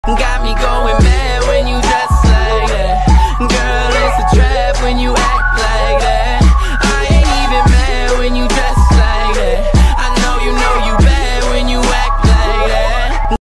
like that shot animation test(loop)
I use the mouse to animate this thing

I'm may or may not keep the characters